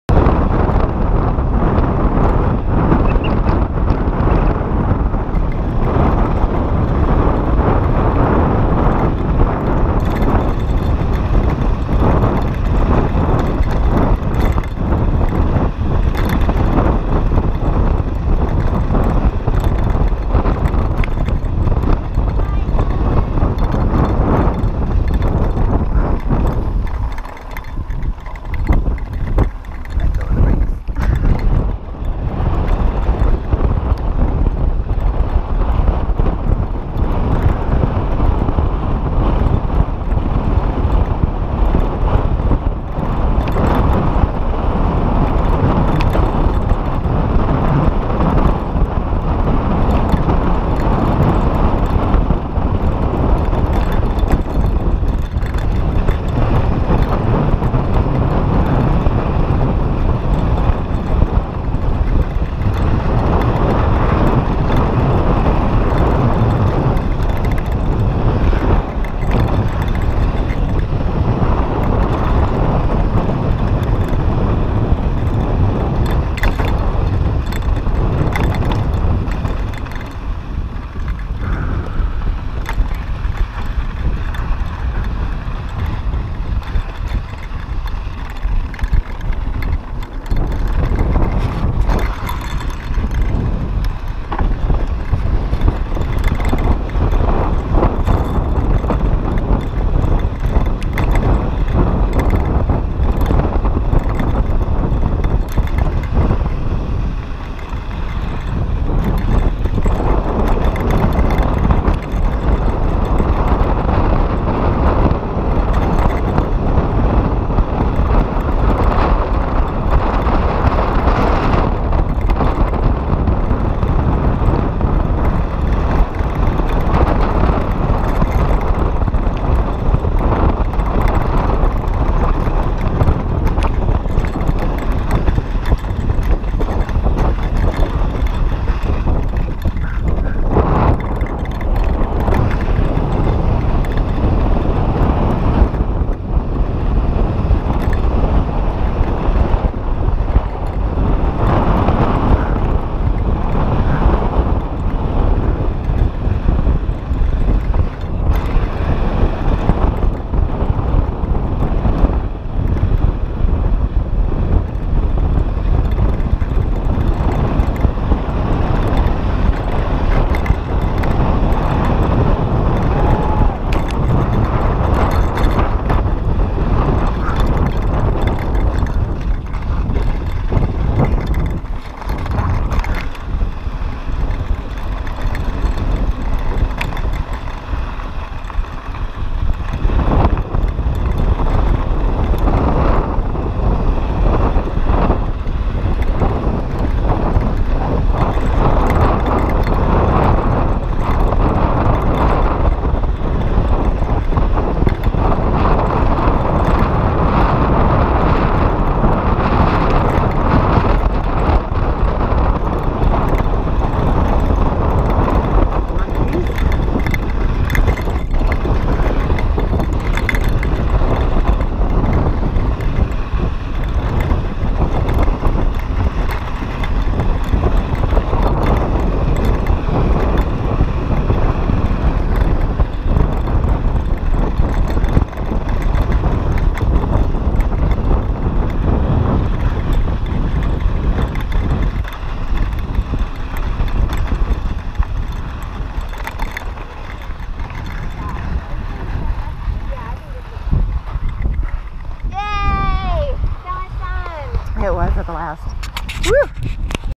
Mammoth 'Off The Top'
Fun Women's 3-day Mountain Biking Clinic w/ Leigh Donovan 2016